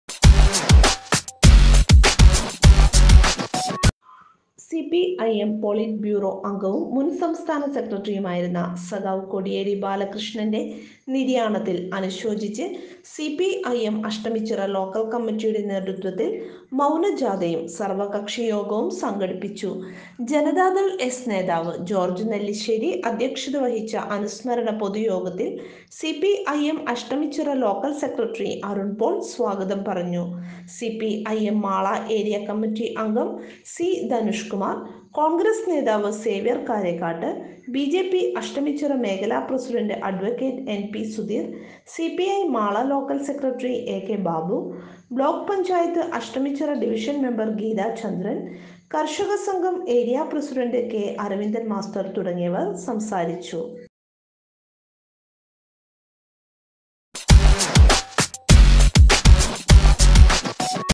കോടിയേരി ബാലകൃഷ്ണന്റെ നിര്യാണത്തിൽ അനുശോചിച്ച് സിപിഐഎം അഷ്ടമിച്ചിറ ലോക്കൽ കമ്മിറ്റി
സിപിഐഎം പൊളിറ്റ് ബ്യൂറോ അംഗവും മുൻ സംസ്ഥാന സെക്രട്ടറിയുമായിരുന്ന സഖാവ് കോടിയേരി ബാലകൃഷ്ണന്റെ നിര്യാണത്തിൽ അനുശോചിച്ച് സിപിഐഎം അഷ്ടമിച്ചിറ ലോക്കൽ കമ്മിറ്റിയുടെ നേതൃത്വത്തിൽ മൗന ജാഥയും സർവ്വകക്ഷി യോഗവും സംഘടിപ്പിച്ചു. ജനതാദൾ എസ് നേതാവ് ജോർജ് നെല്ലിശ്ശേരി അധ്യക്ഷത വഹിച്ച
അനുസ്മരണ പൊതുയോഗത്തിൽ
സിപിഐഎം അഷ്ടമിച്ചിറ ലോക്കൽ സെക്രട്ടറി അരുൺ പോൾ സ്വാഗതം പറഞ്ഞു.
സിപിഐഎം  മാള ഏരിയ കമ്മിറ്റി അംഗം  സി ധനുഷ് കുമാർ, കോൺഗ്രസ് നേതാവ് സേവ്യർ കാരേക്കാട്ട്, ബിജെപി അഷ്ടമിച്ചിറ മേഖല പ്രസിഡൻറ് അഡ്വക്കേറ്റ് എൻ പി സുധീർ, സിപിഐ മാള ലോക്കൽ സെക്രട്ടറി എ കെ ബാബു, ബ്ലോക്ക് പഞ്ചായത്ത് അഷ്ടമിച്ചിറ ഡിവിഷൻ  മെമ്പർ ഗീത ചന്ദ്രൻ, കർഷക സംഘം ഏരിയാ പ്രസിഡൻറ് കെ അരവിന്ദൻ മാസ്റ്റർ തുടങ്ങിയവർ സംസാരിച്ചു